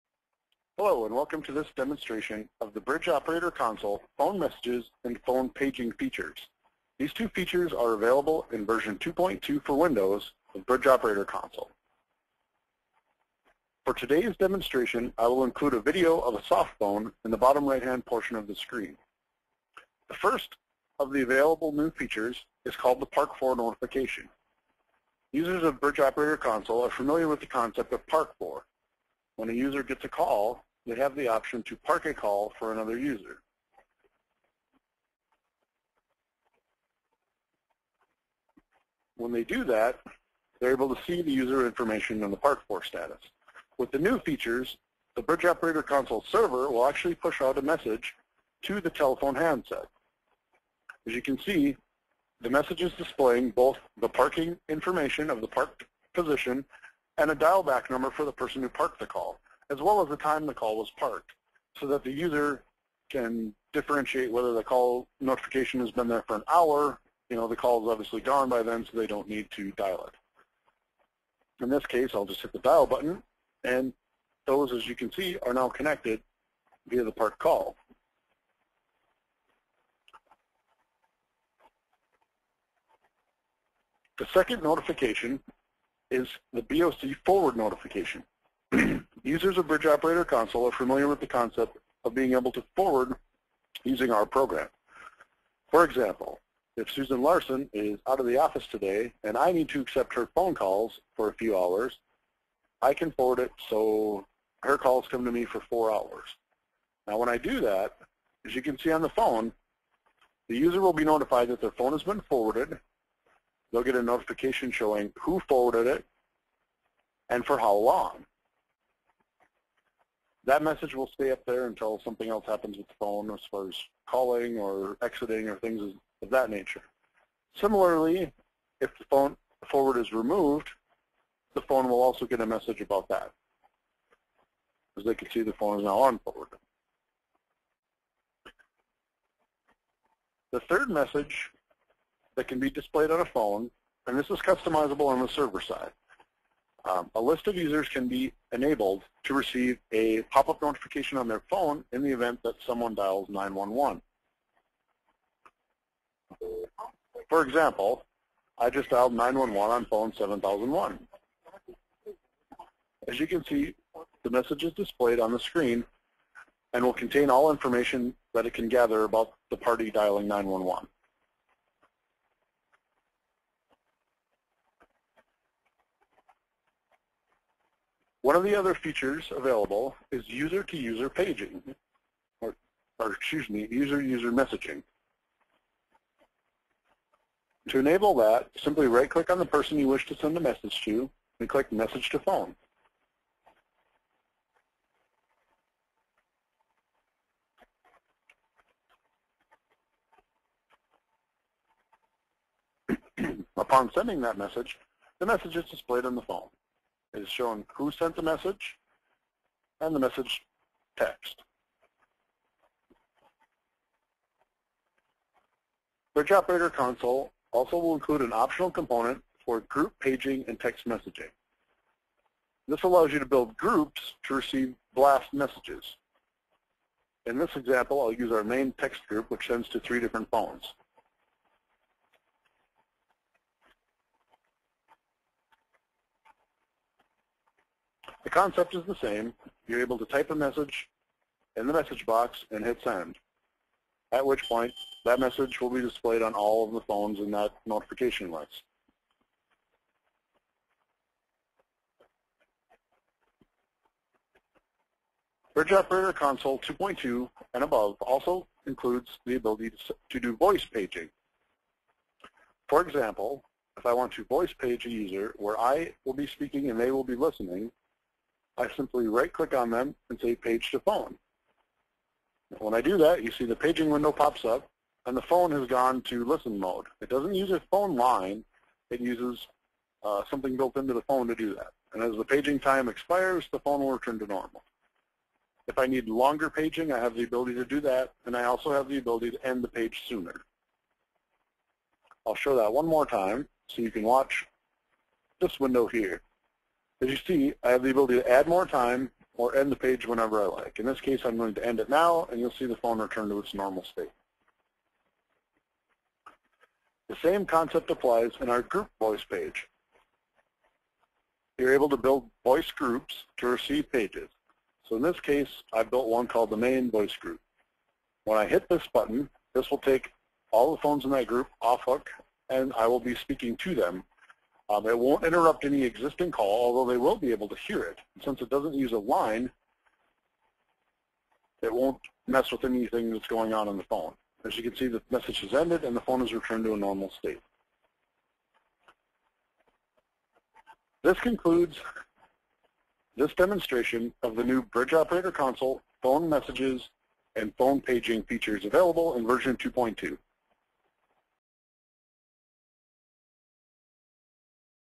Bridge Operator Console - Phone Messaging and Paging (Replacement for Cisco Attendant Console)
A video demostration of the messaging and paging capabilities built into version 2.2 of Bridge Operator Console for Windows